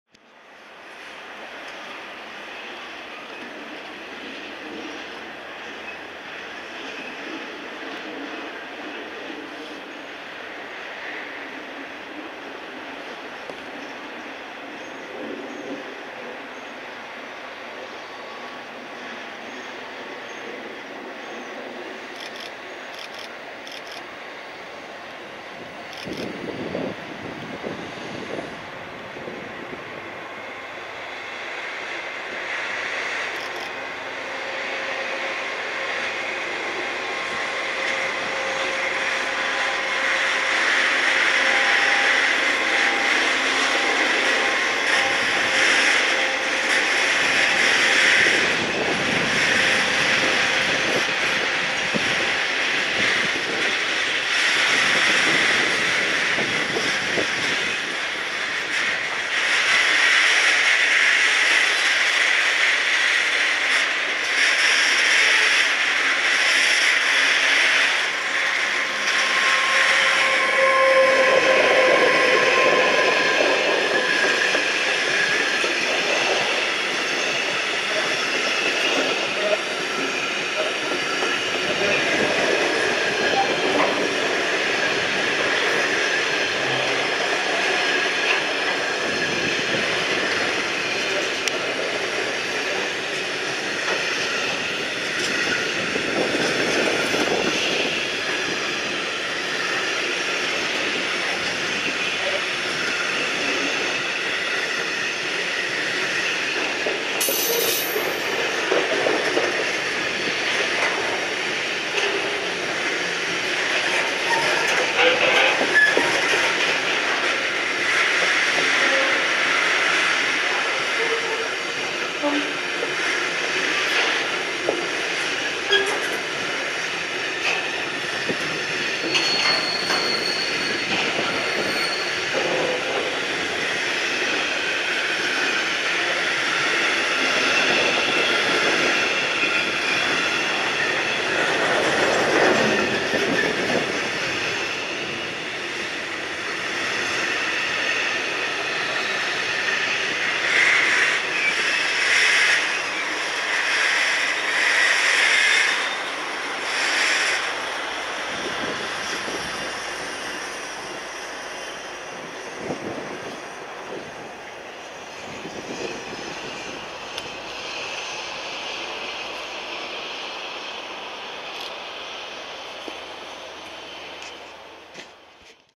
4K RBH 811 mit Kohlezug und VTG/Retrack 189 211 mit Containerzug bei Oberhausen-West (DE) (5-1-2022)
(DE) RBH 811 mit Kohlezug und VTG/Retrack 189 211 mit Containerzug bei Oberhausen-West

(NL) RBH 811 met kolentrein en VTG/Retrack 189 211 met containertrein bij Oberhausen-West

(EN) RBH 811 with coiltrain and VTG/Retrack 189 211 with containers at Oberhausen-West